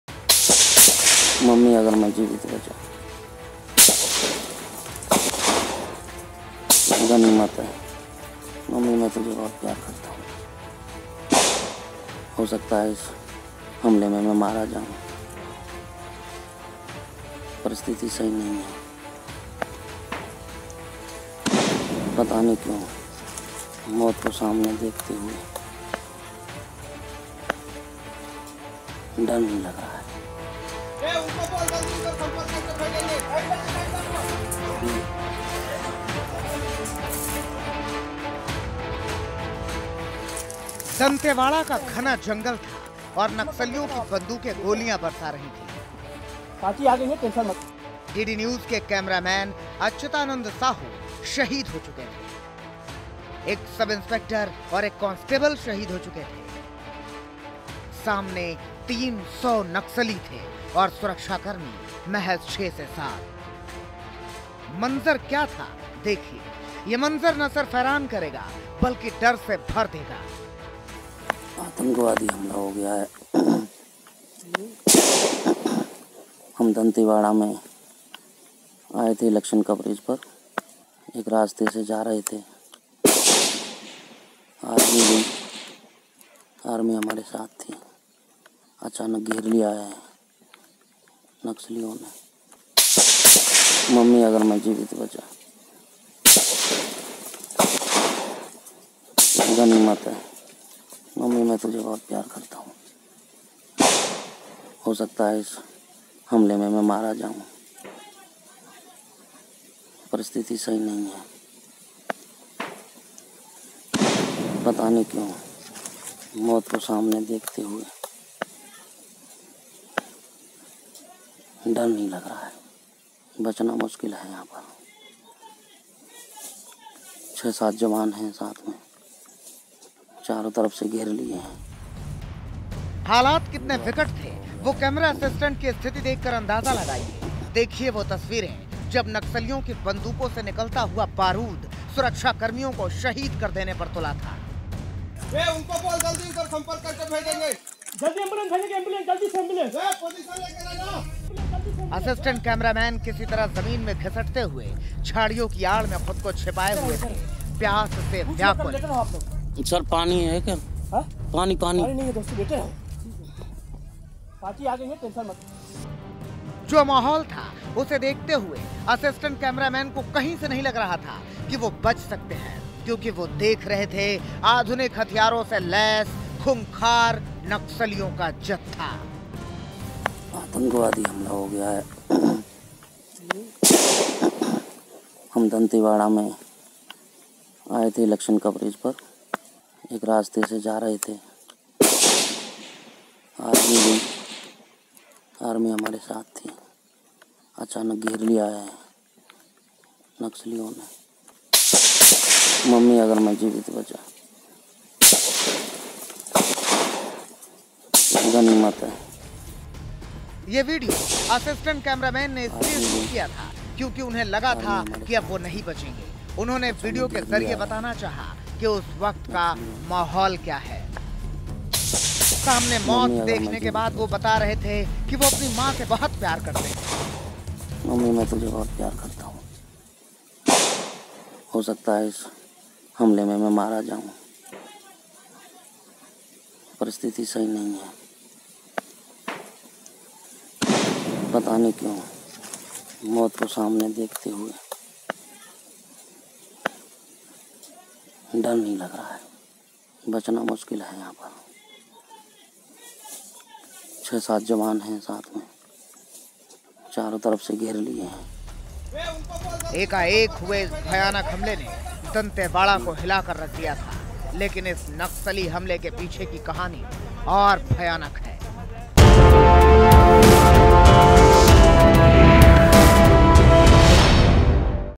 छत्तीसगढ़ में हुए नक्सली हमले का वीडियो आया सामने| News Tak
मम्मी अगर मैं जीवित बचा, गनीमत है। मम्मी मैं तुझे बहुत प्यार करता हूं। हो सकता है इस हमले में मैं मारा जाऊं। परिस्थिति सही नहीं है। पता नहीं क्यों? मौत को सामने देखते हुए डर नहीं लग रहा है। बचना मुश्किल है यहां पर... ये शब्द हैं उस मीडियाकर्मी के, जिसने नक्सलियों के रूप में मौत को सामने से देखा है, यही नहीं उन्होंने अपने मोबाइल से एक वीडियो भी बनाया है जिसमें वो अपना डर बयां कर रहे हैं। गनीमत यह है कि अपनी कहानी बताने को ये मीडियाकर्मी जिंदा है और दैनिक जागरण ने मोर मुकुट शर्मा नामक इस मीडियाकर्मी से फोन पर बात की।

Mom, I love you... I might die today... But somehow I am not scared even though I am facing death. These heartbreaking words were part of Doordarshan staffer Mormukut Sharma's message that he recorded while being attacked by Naxals in Chhattisgarh's Dantewada. Sharma's colleague -- cameraman Achyutananda Sahu -- died in that attack. As did two security personnel. Moments after the Doordarshan crew came under attack Sharma, a lighting assistant with the state-run broadcaster, whipped out his phone and recorded a message for his mother. About News Tak (न्यूज़ तक):